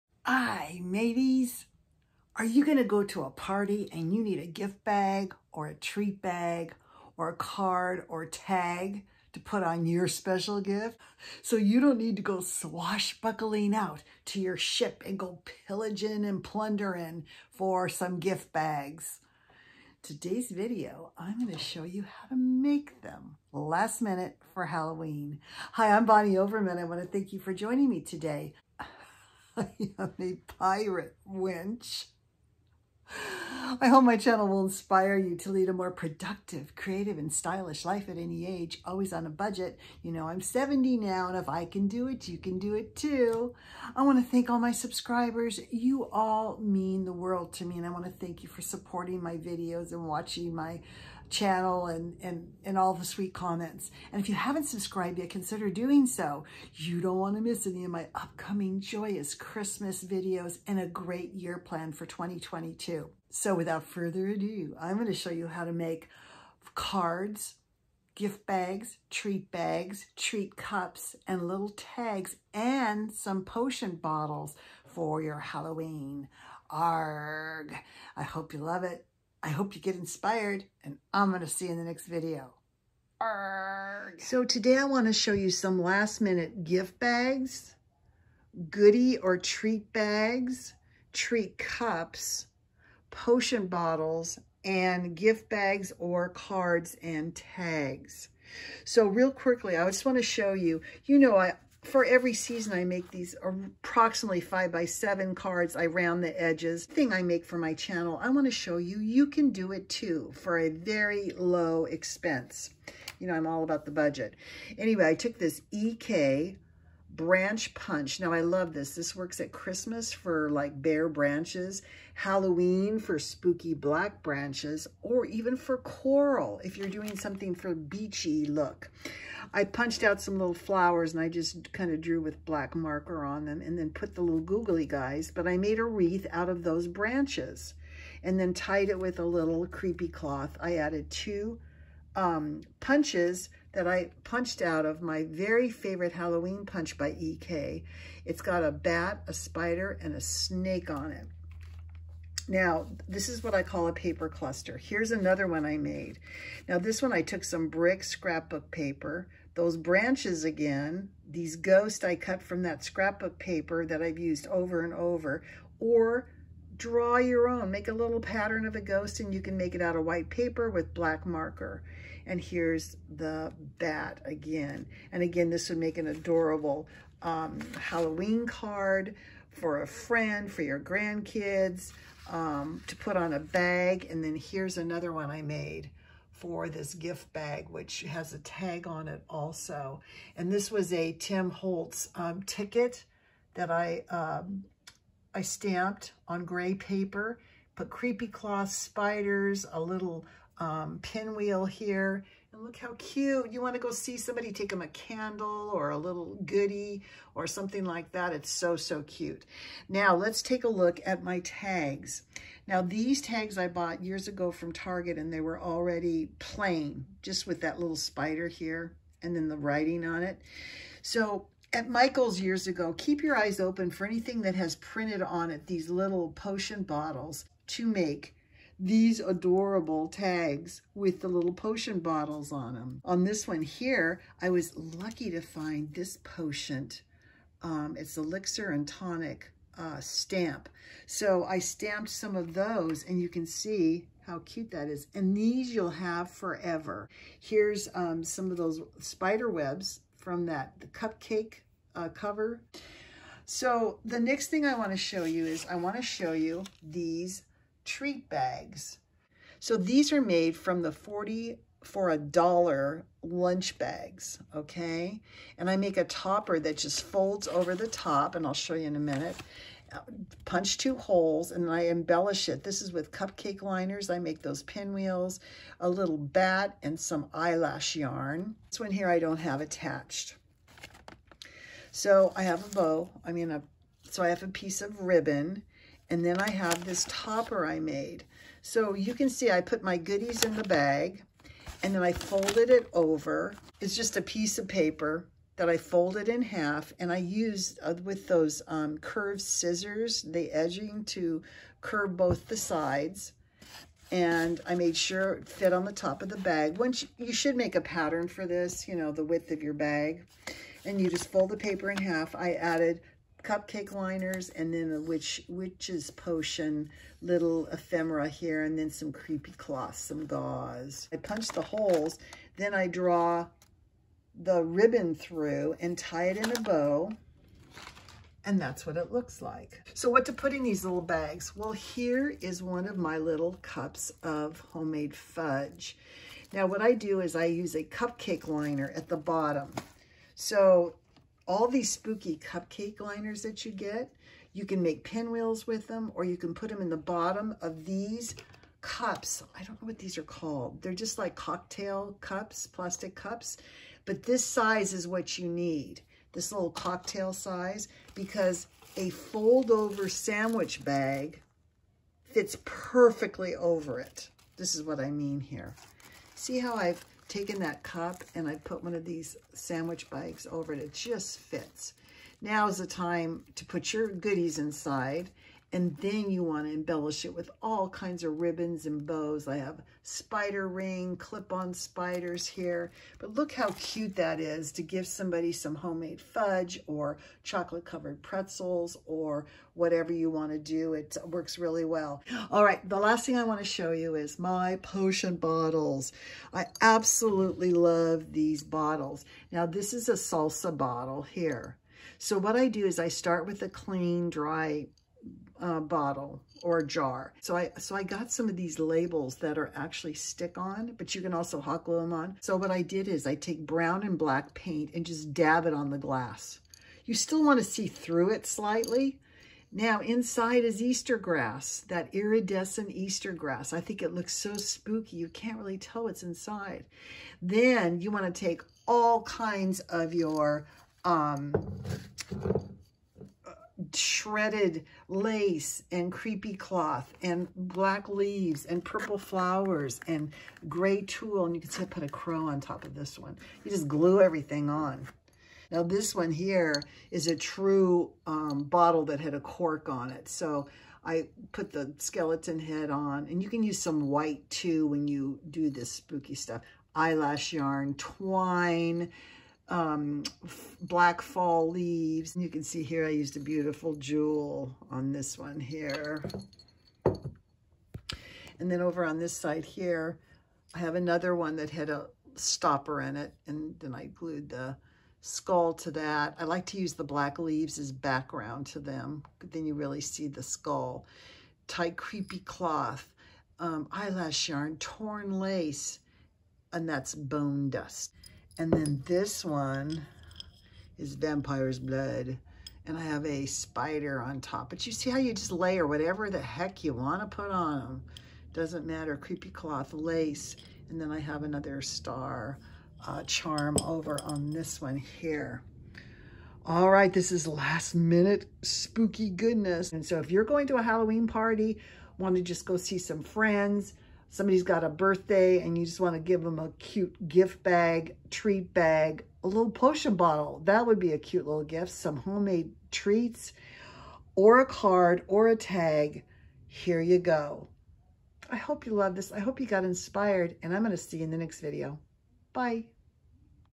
Potion bottles and gift bags
Today I’ll share some last minute crafts for Halloween. Gift and treat bags, cards, tags and potion bottles🧡🖤